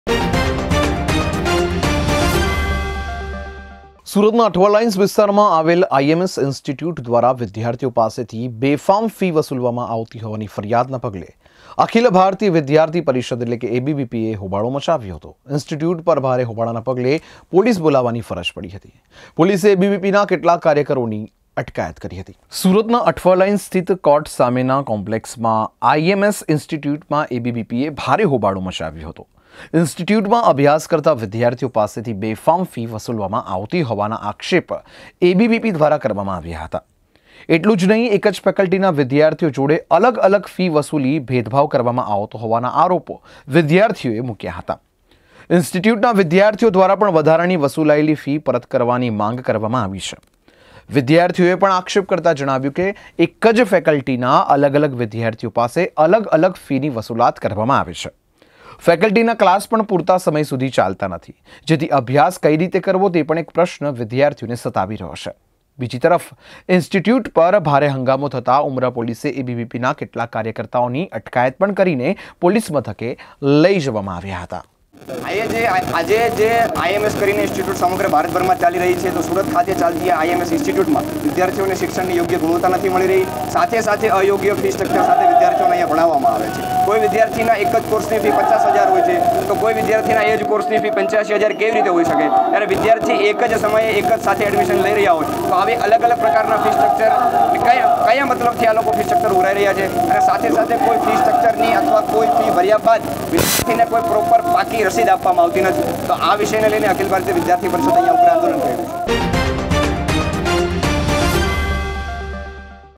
ખાનગી ઈન્સ્ટિટ્યૂટ દ્વારા વિદ્યાર્થીઓ પાસેથી બેફામ ફી વસુલાતી હોવાના આક્ષેપ સાથે હોબાળો | G One News
સુરતના અઠવાલાઇન્સ વિસ્તારમાં આવેલ IMS ઇન્સ્ટિટ્યૂટ દ્વારા વિધાર્થીઓ પાસેથી બેફામ ફી વસૂલવામાં આવતી હોવાની ફરિયાદના પગલે અખિલ ભારતીય વિધાર્થી પરિષદ(ABVP)એ હોબાળો મચાવ્યો હતો. ઇન્સ્ટિટયૂટ પર ભારે હોબાળાના પગલે પોલીસ બોલાવવી પડી હતી. પોલીસે એબીવીપીના કેટલાક કાર્યકરોની અટકાયત કરી હતી. G-onenews